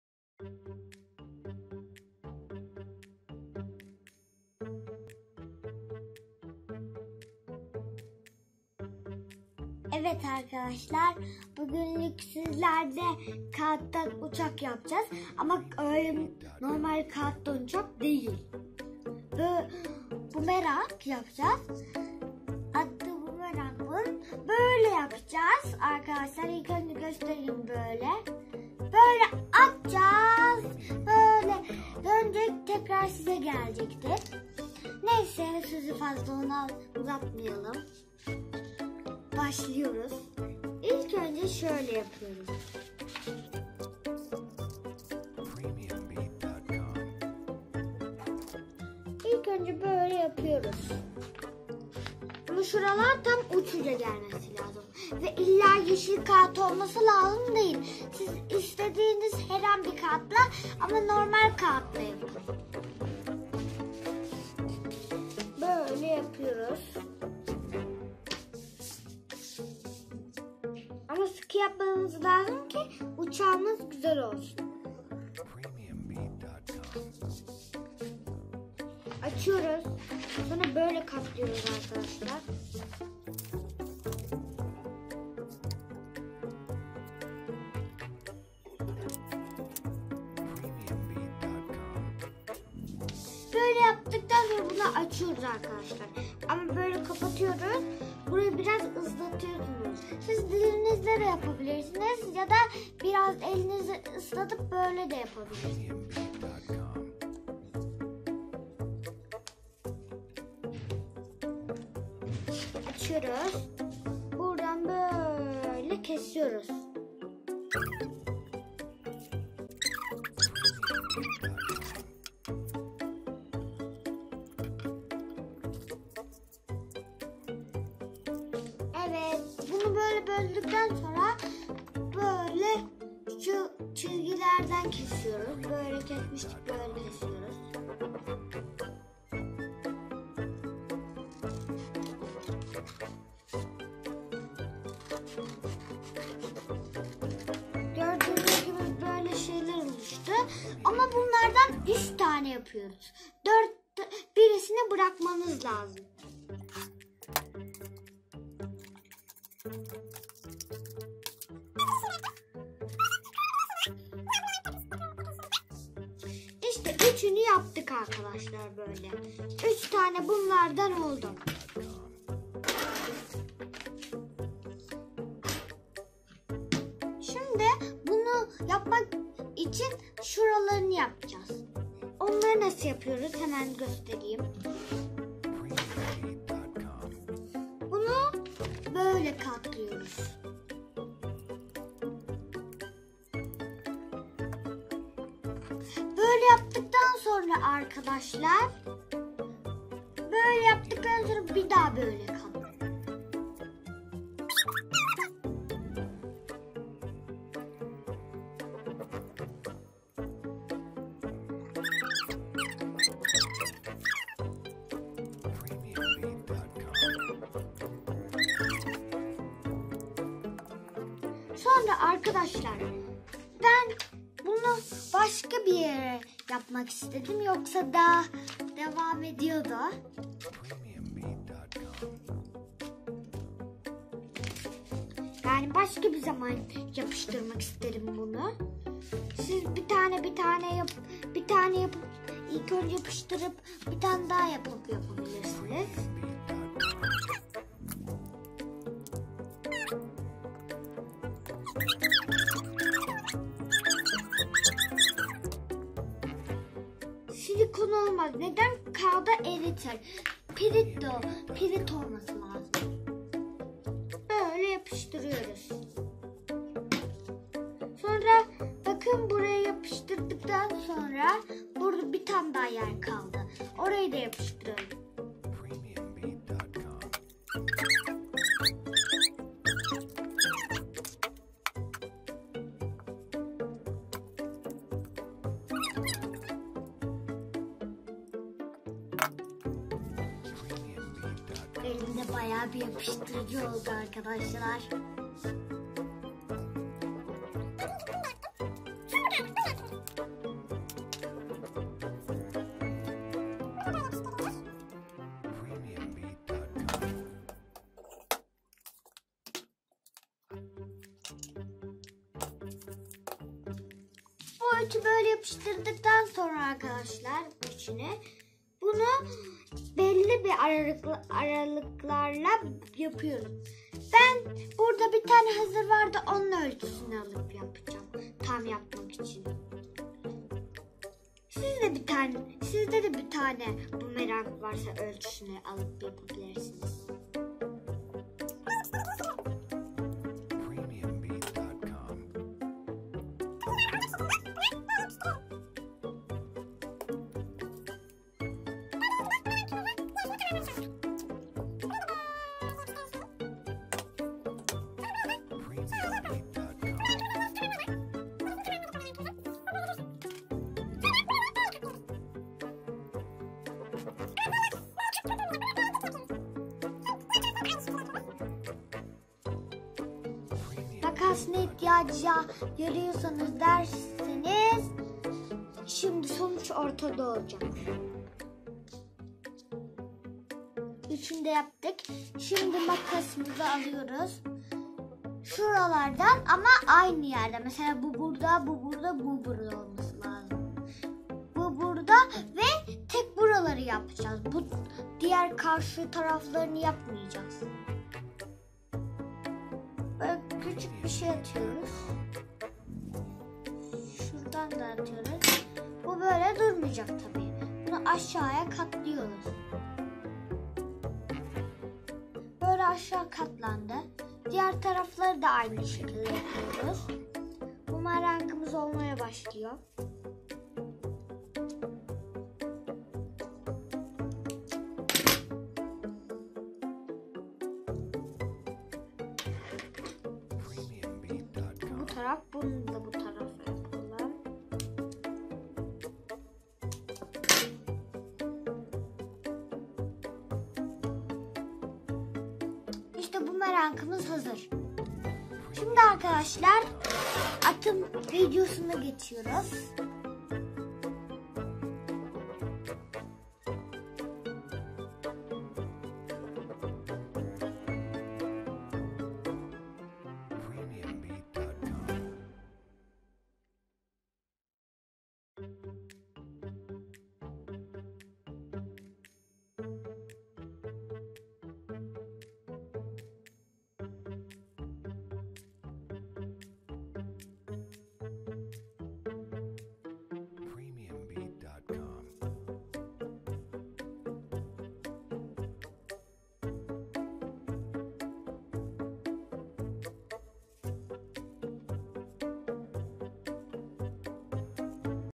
Alper Tunga İle Çok Kolay BUMERANG Yapımı
istanbul günaydın Çocuklar için harika bir etkinlik. Evde Çocukların ve Büyüklerin Yapabileceği çok kolay bir Etkinlik